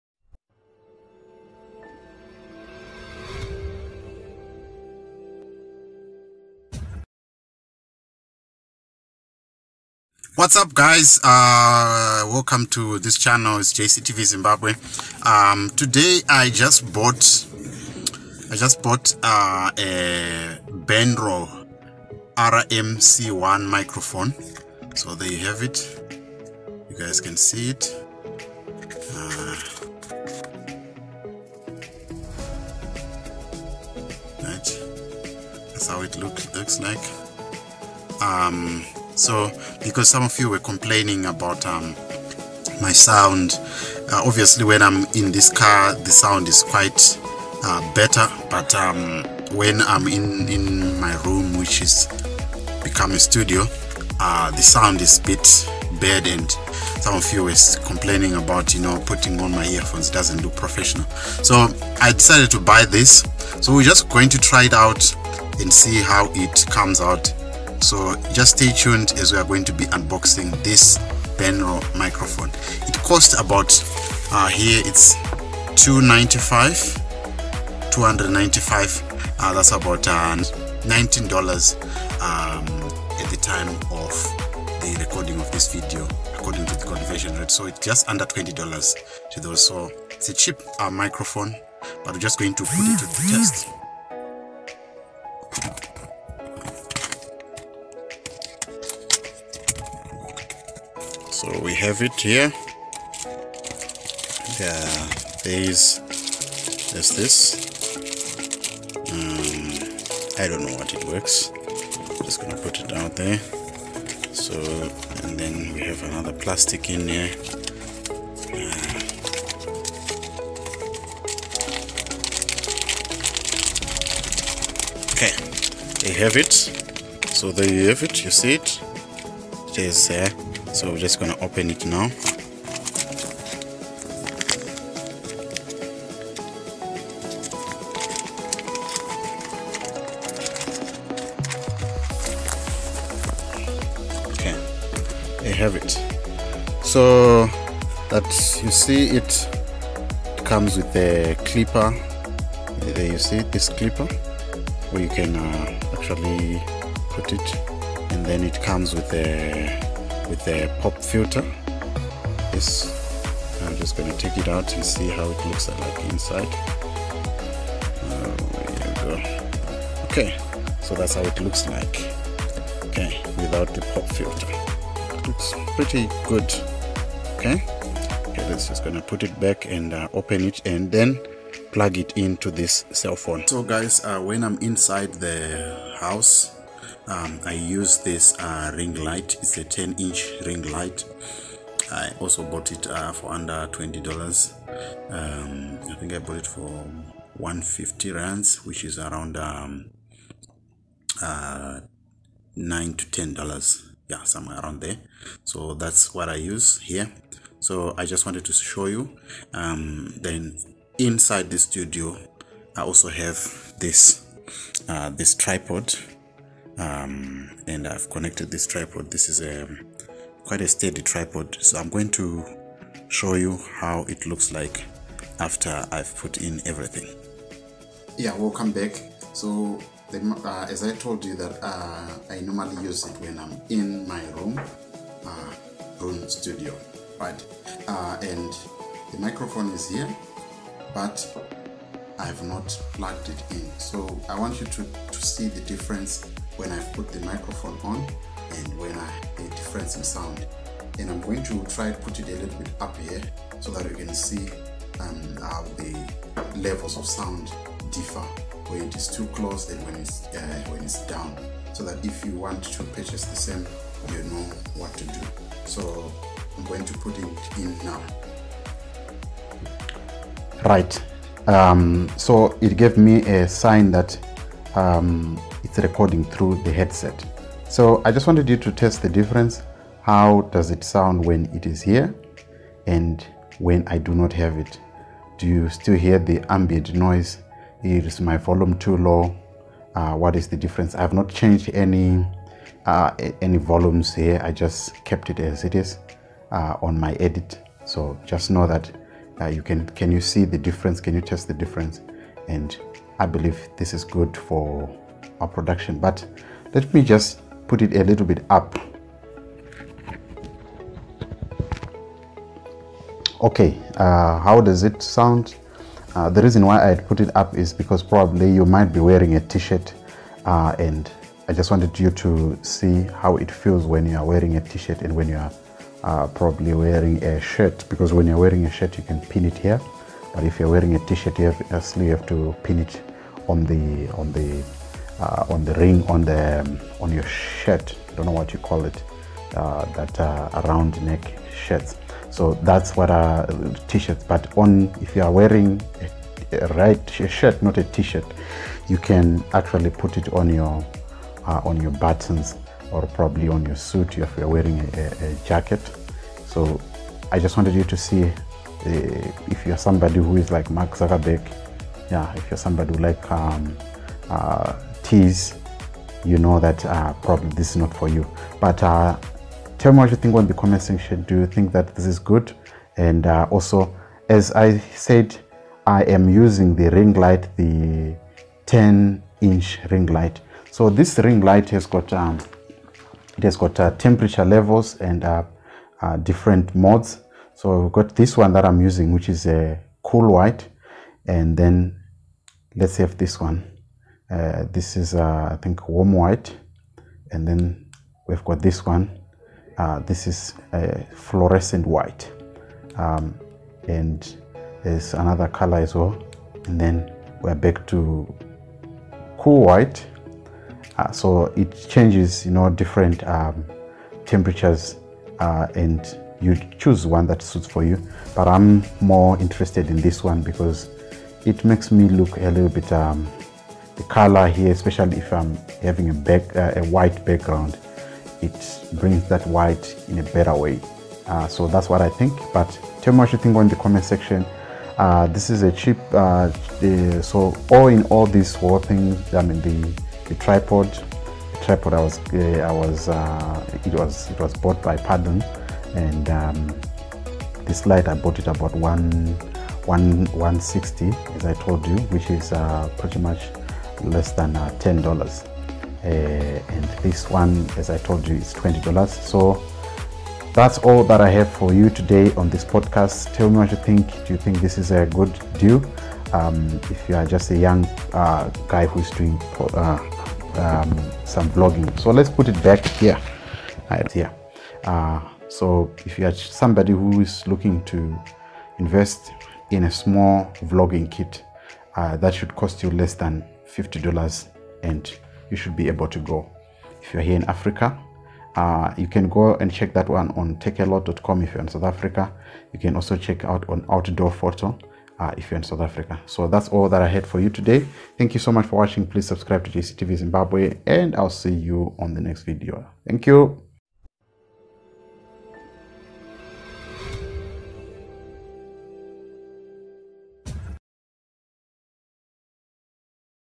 AMAZING! Best Android microphone for vlogging/ Unboxing BENRO RMC1
I have had complaints about my sound not being perfect in my room so I decided to get better quality. This mic is better than earphones
Please donate for me to get more equipment  on PayPal 2